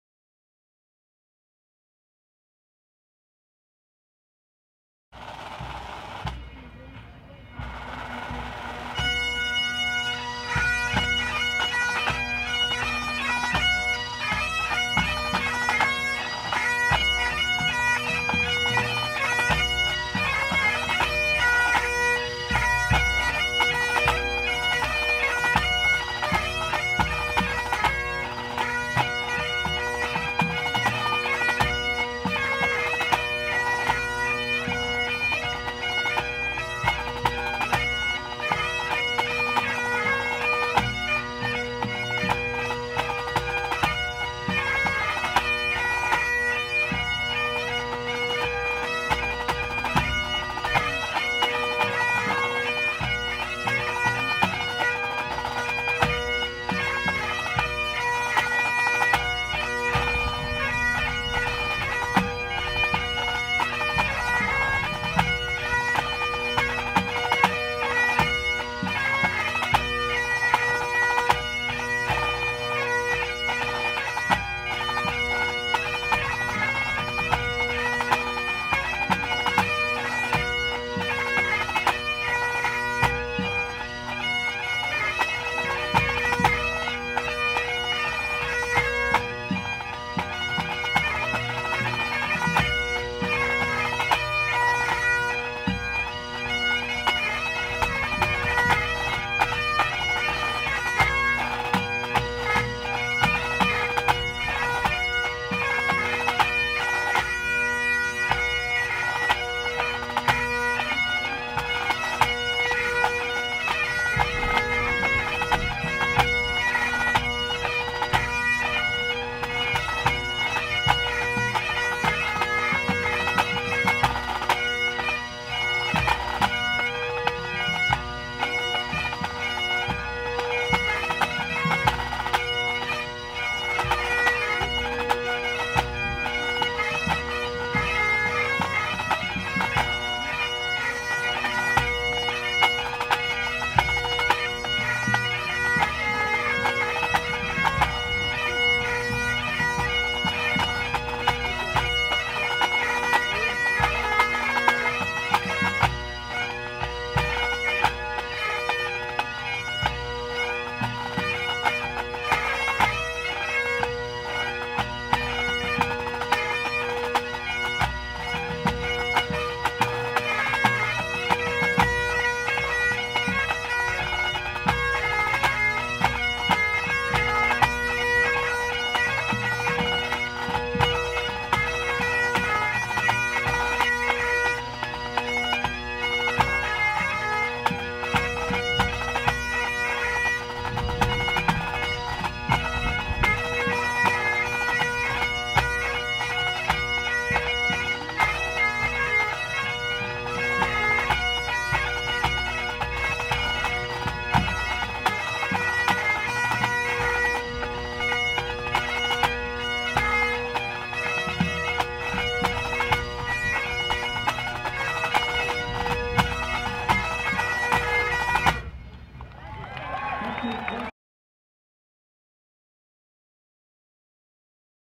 06 Closkelt
Recorded live at Glasgow Green on 17th August, the 2019 Grade 2 Final of the World Pipe Band Championships